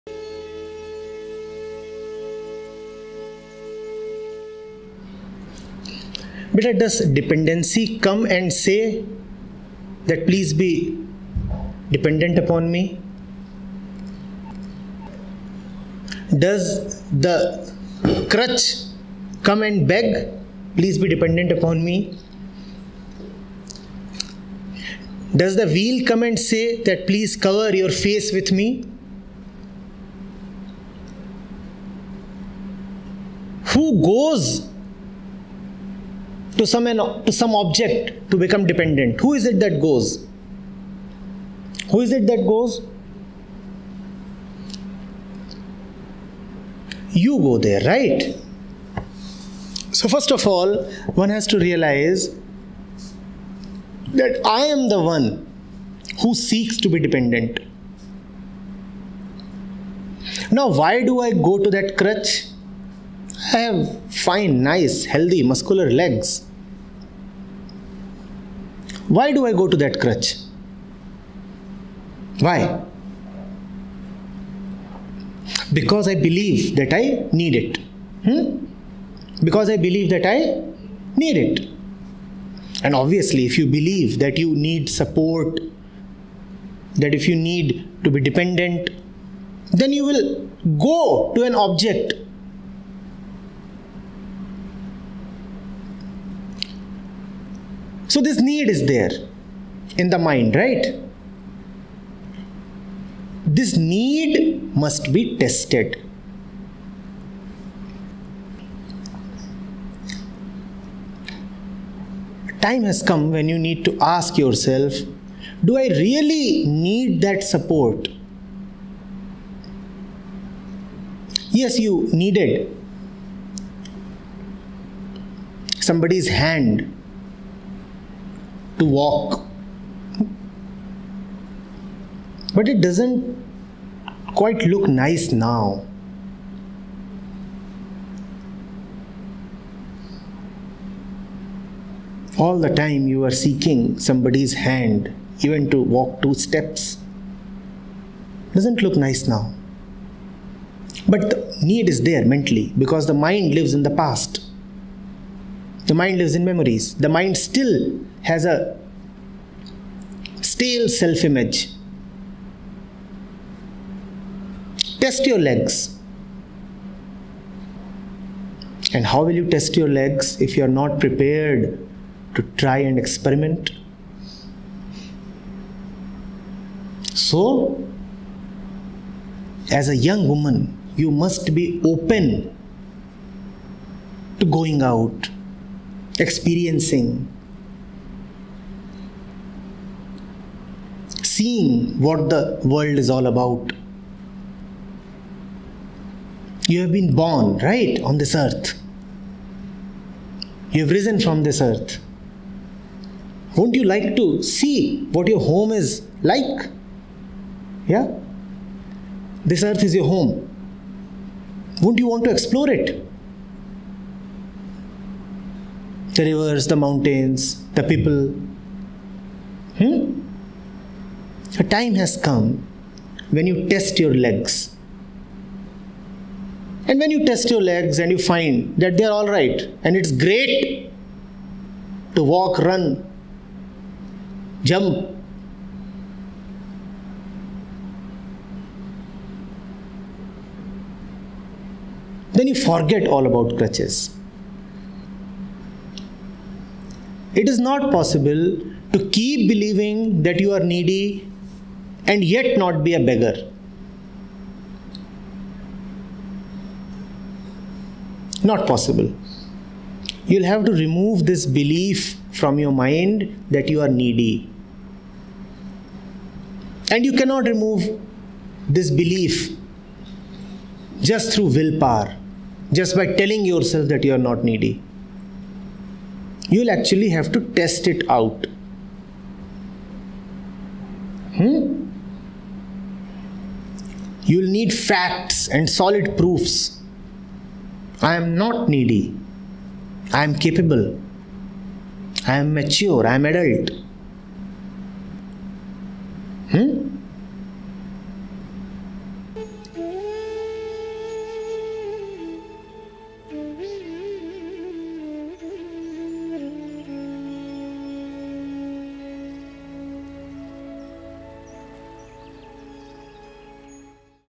How to get rid of dependencies in life? || Acharya Prashant, with youth (2015)
Video Information:
Samvaad session, 06.04.15, Gurgaon, Uttar pradesh, India

Context:
Why we are so Dependent on Someone or Something?
What are our Needs?
How to become Independent?